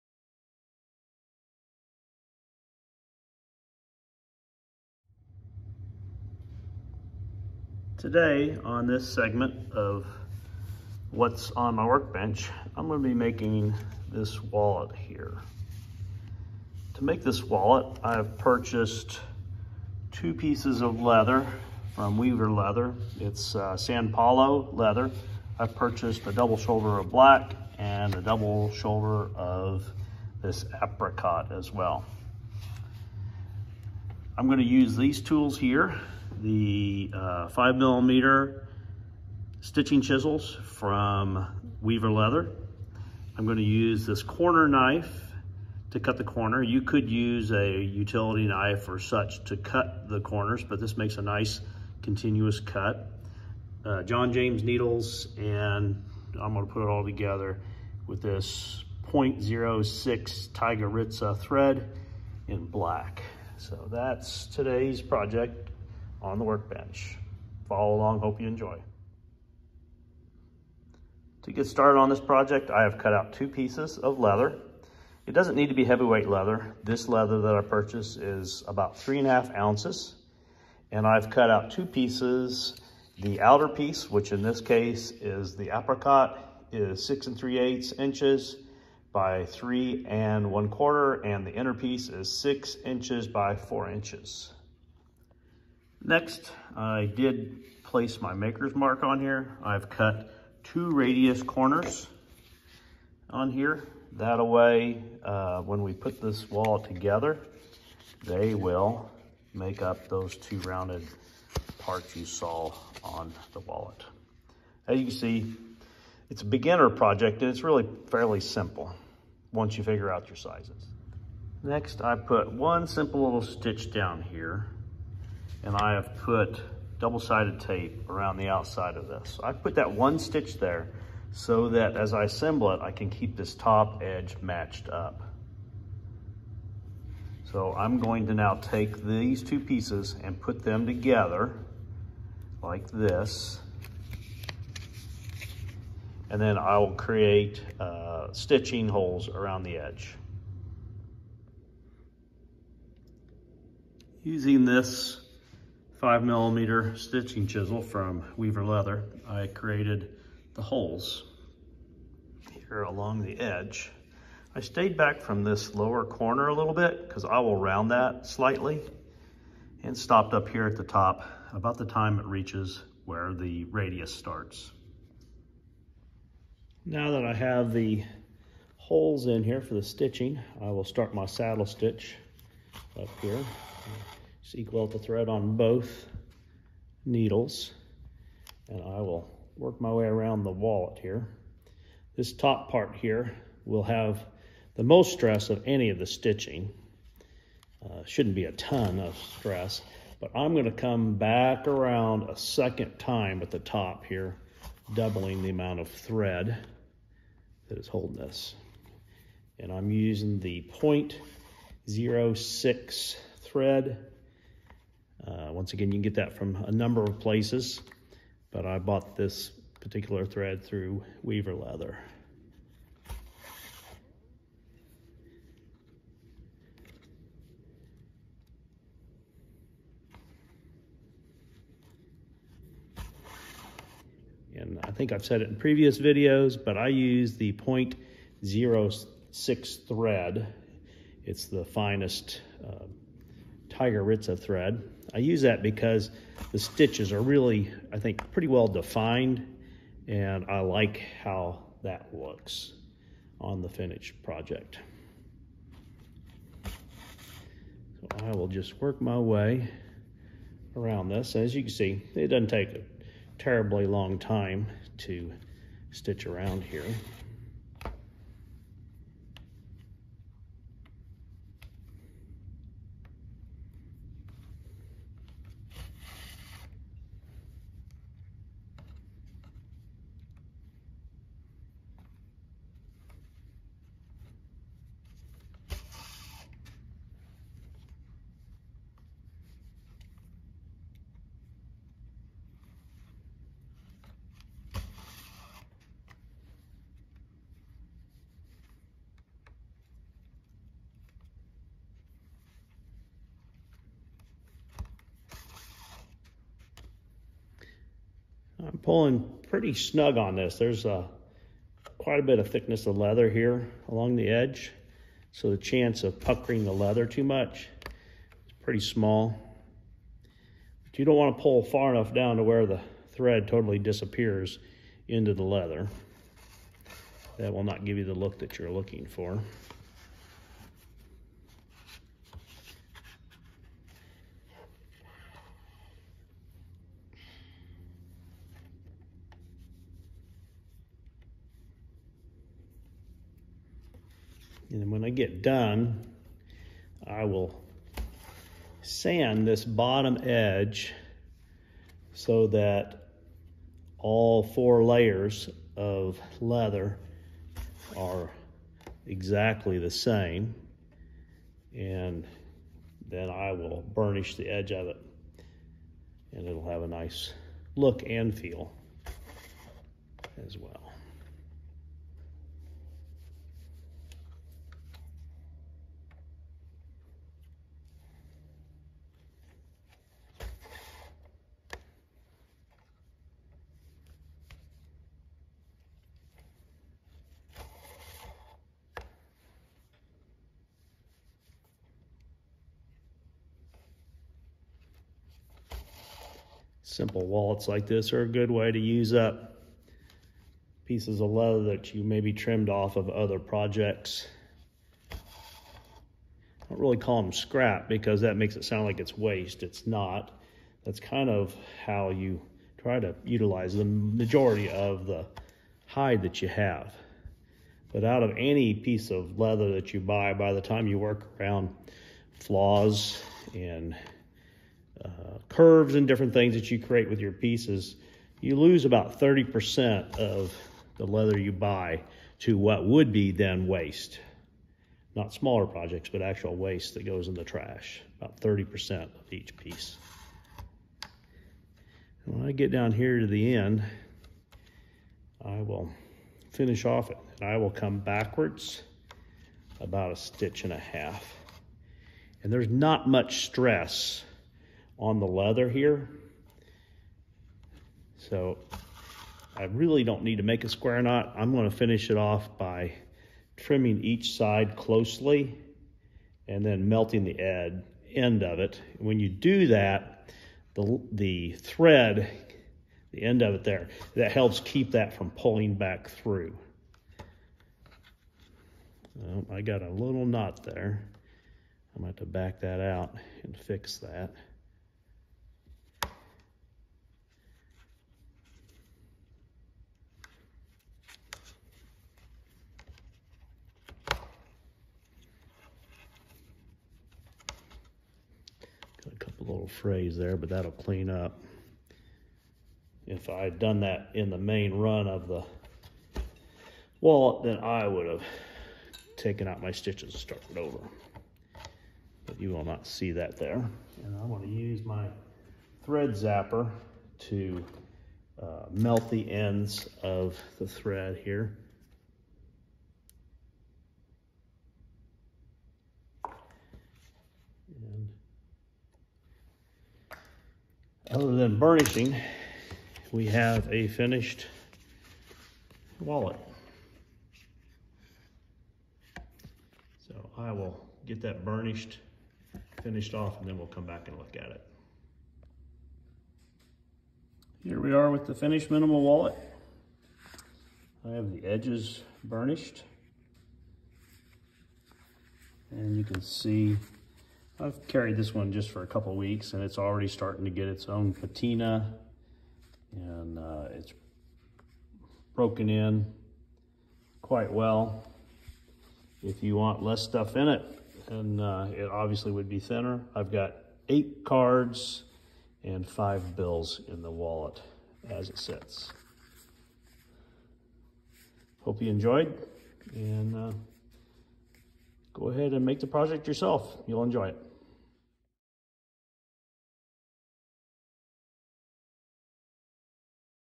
How to make a Minimalist Wallet @InMyBusyLittleShop
This simple but stylish wallet Minimal Wallet@InMyBusyLittleShop was made from items purchased at weaverleather This is a great beginner project that you can complete in an afternoon. Follow along and make this wallet from leather that you have leftover from other projects. It could also be made from leather that was used to make a purse or other item, making a nice matching item. I hope you enjoy.